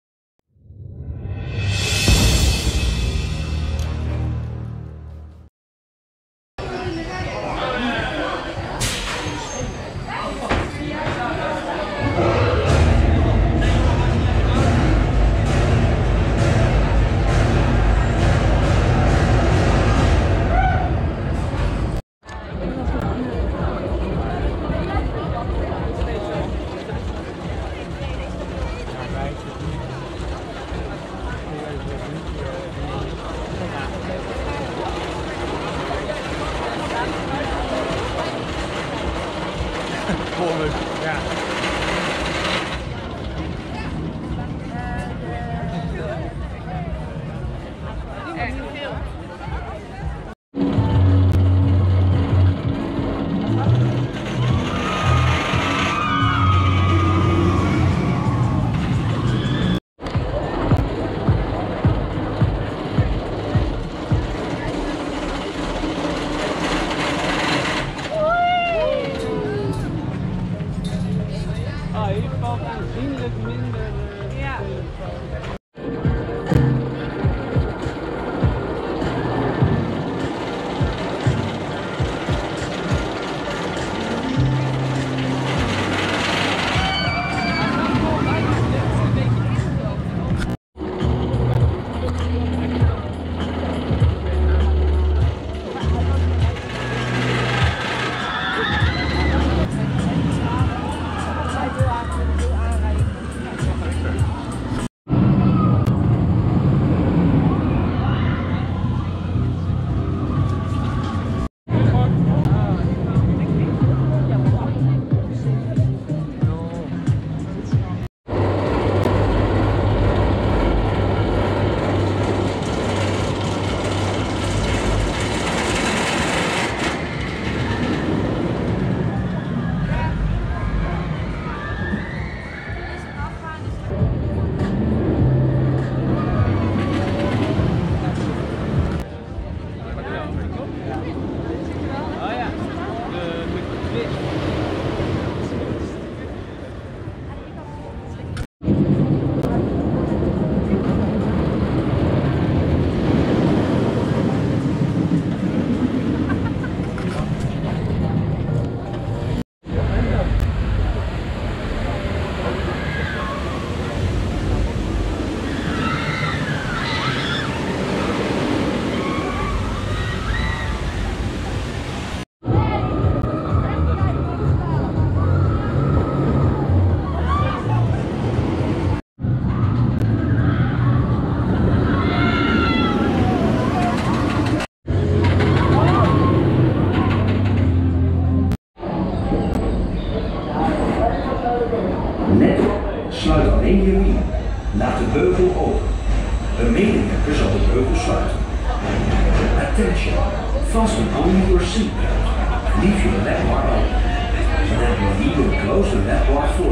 Untamed - NEW 2019 - Offride - Walibi Holland - RMC I-BOX Hybid
Untamed im Walibi Holland ein Hybrid umbau durch Rocky Mountain Construction (RMC) von Robin Hood